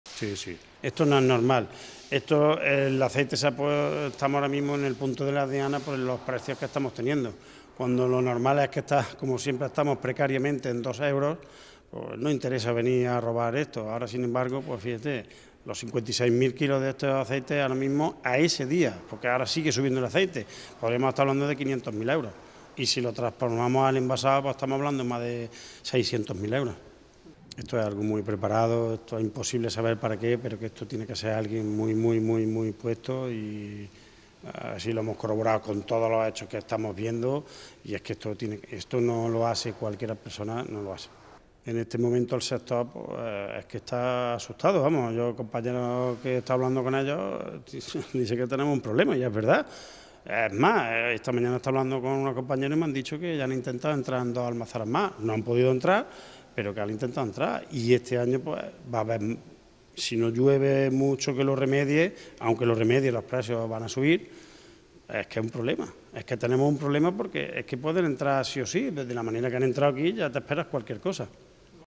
La almazara con 56.000 litros de aceite robados teme que vuelvan a entrarle por la subida de pr
El gerente de la almazara Marín Serrano El Lagar, del municipio cordobés de Carcabuey, que el miércoles ha sufrido el robo de 56.000 litros de aceite de oliva virgen extra, ha dicho que teme que vuelva a producirse un robo similar por la subida del precio del aceite, de modo que "en este momento el sector está asustado" y "tenemos un problema".